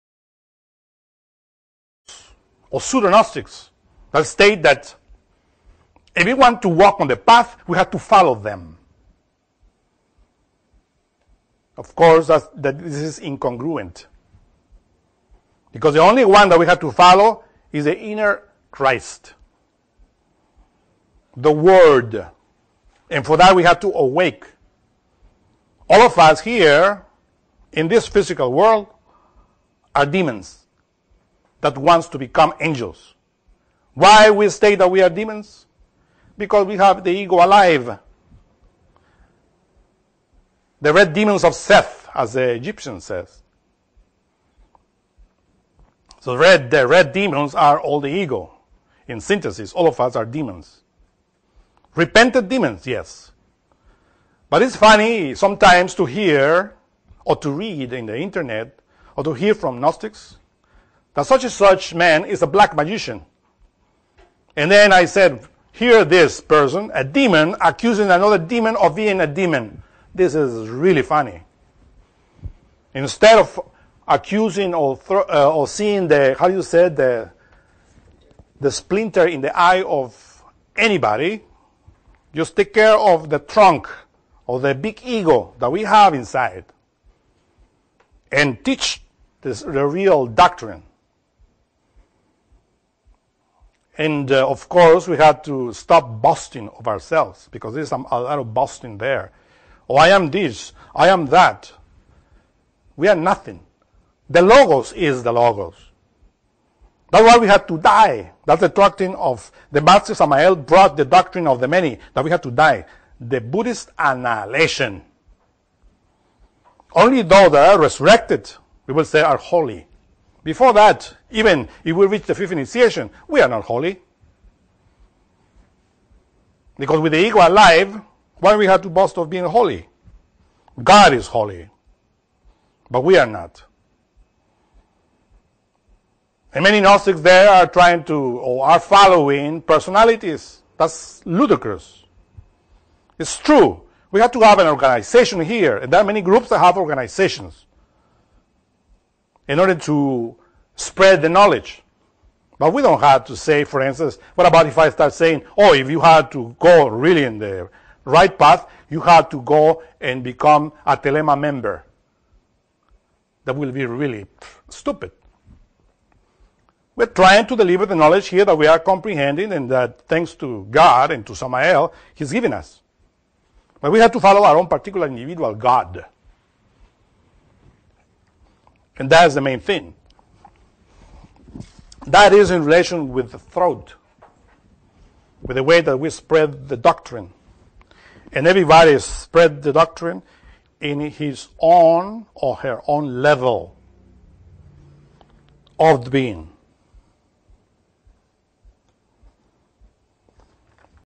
Demons Accusing Demons of Being Demons
Excerpt from lecture titled "Book of Revelation 07: Church of Sardis"

1 Judge not, that ye be not judged. 2 For with what judgment ye judge, ye shall be judged: and with what measure ye mete, it shall be measured unto you. 3 And why beholdest thou the mote that is in thy brother's eye, but considerest not the beam that is in thine own eye? 4 Or how wilt thou say to thy brother, Let me cast out the mote out of thine eye; and lo, the beam is in thine own eye? 5 Thou hypocrite, cast out first the beam out of thine own eye; and then shalt thou see clearly to cast out the mote out of thy brother's eye. - Matthew 7:1-5